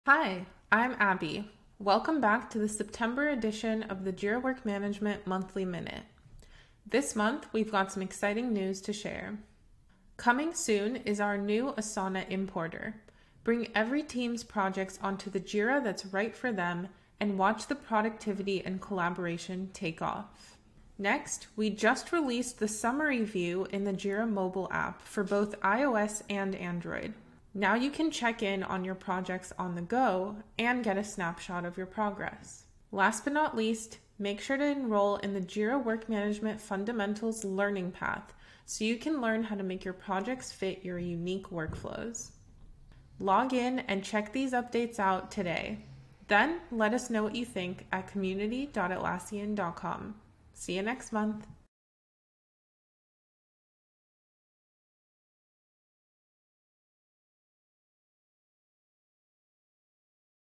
Jira Work Management Monthly Minute: September 2023 | Atlassian
Learn more about the latest Jira Work Management updates in our Monthly Minute.